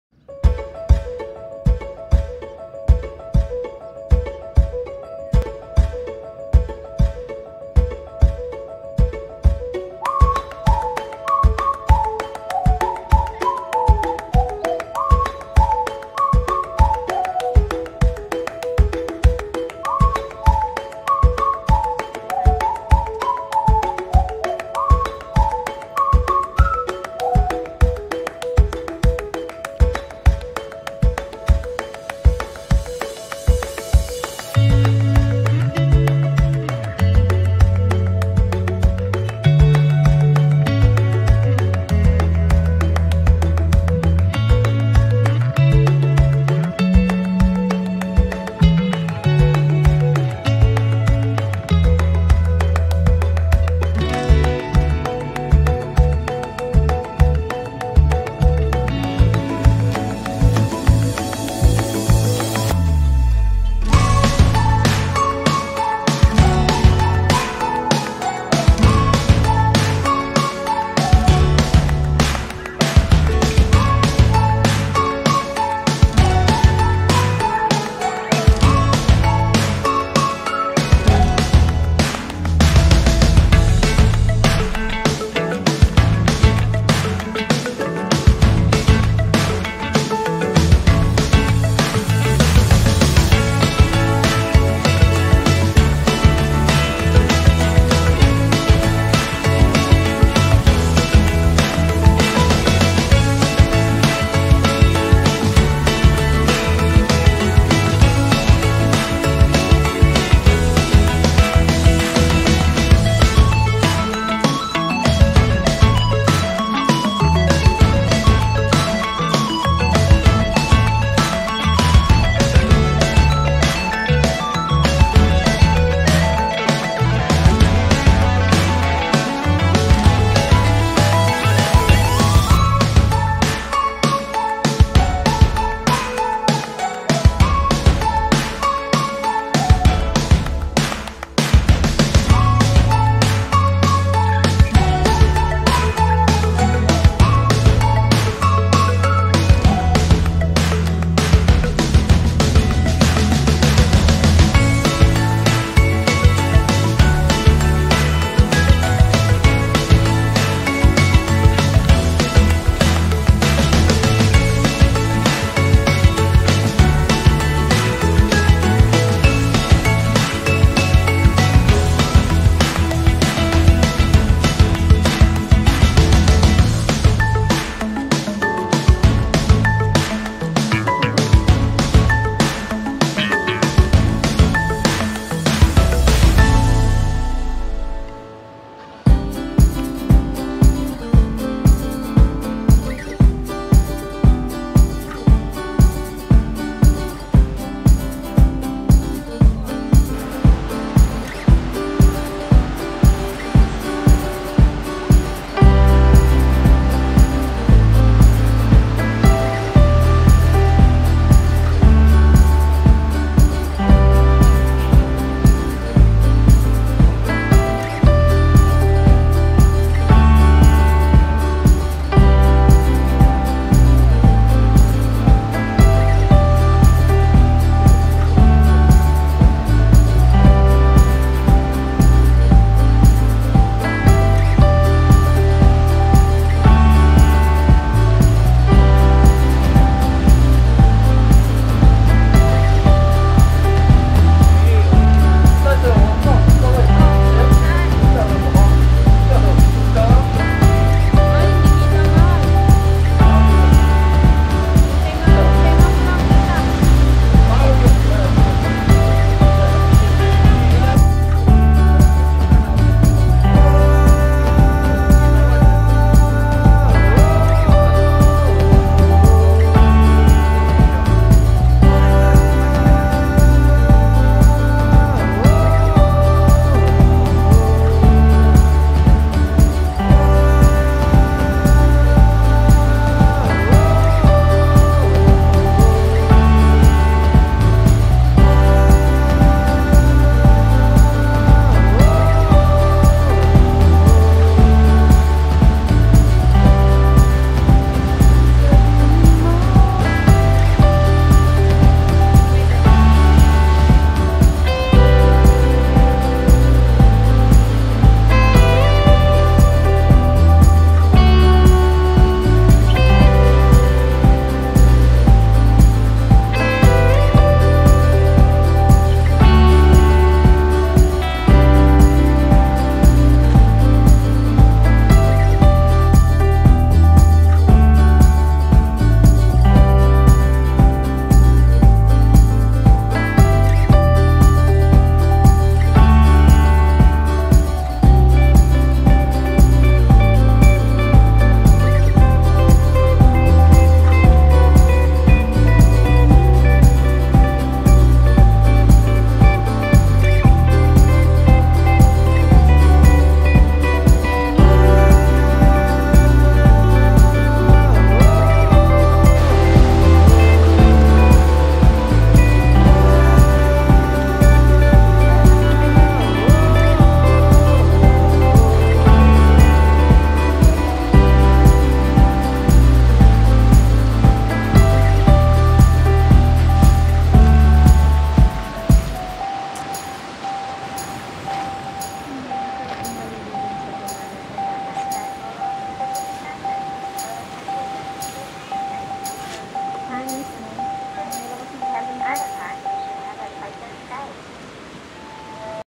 HEPING ISLAND PARK
Ang Heping Island Park ay matatagpuan sa lugar ng KEELUNG (TAIPEI).
Mula sa KEELUNG STATION ay mag taxi po papunta po dun.
Around 15 mins lng po ang byahe at ang pamasahe po ay nsa 200NT.
Meron din pong entrance fee na nagkakahalaga ng 80NT.
Maganda po at magandang kumuha ng mga larawan.